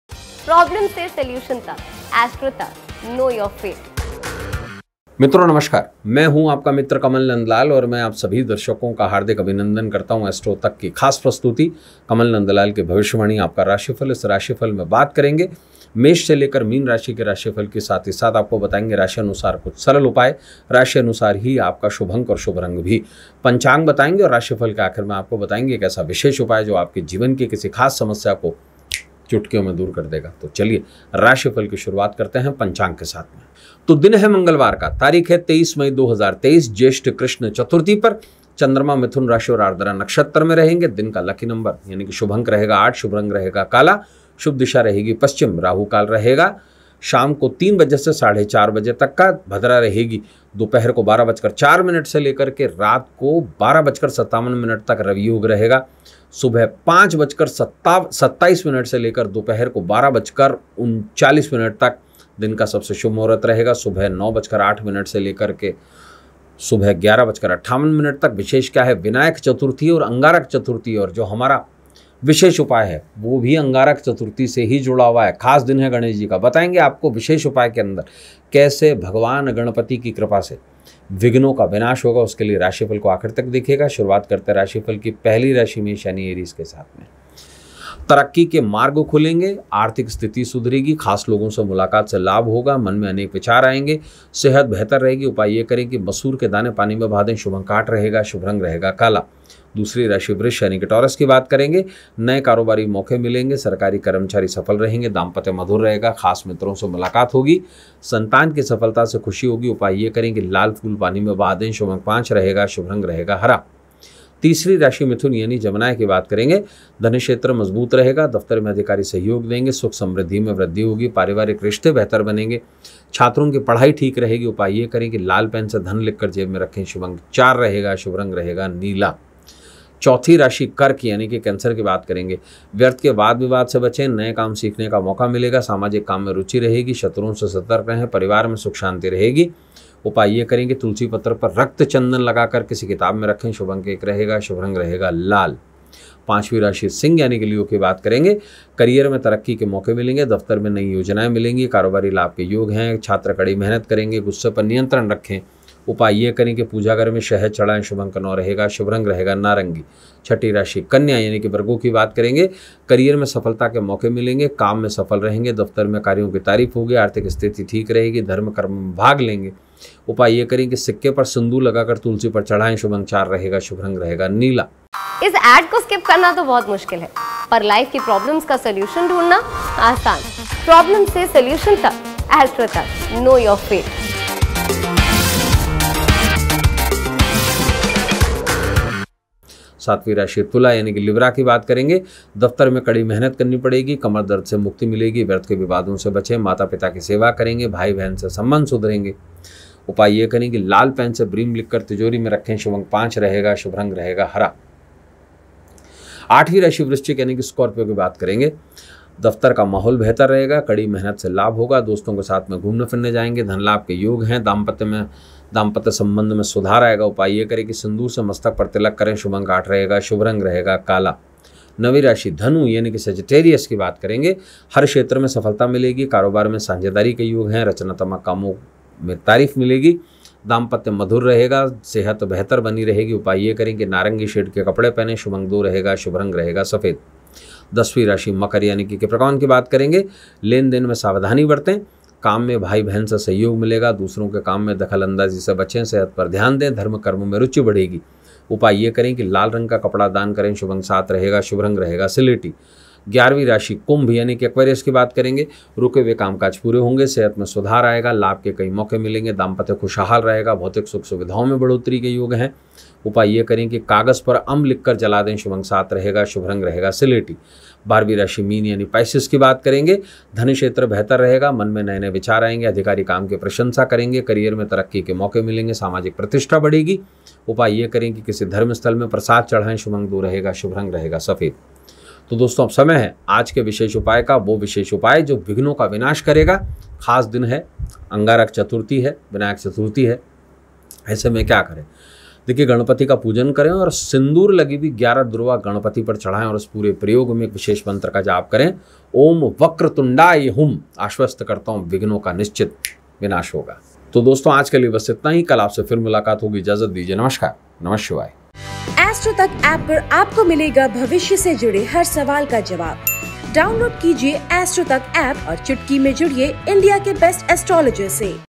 23 मई 2023 | आज का राशिफल | कमल नंदलाल की भविष्यवाणी | Kamal NandLal | Astro Tak
कमल नंदलाल की भविष्यवाणी- 23 मई 2023 : जानिए अपने दिन का राशिफल साथ ही कौन सा रंग है आपके लिए शुभ...किस राशि वालों को क्या उपाय करना चाहिए...सिर्फ कमल नंदलाल की भविष्यवाणी में.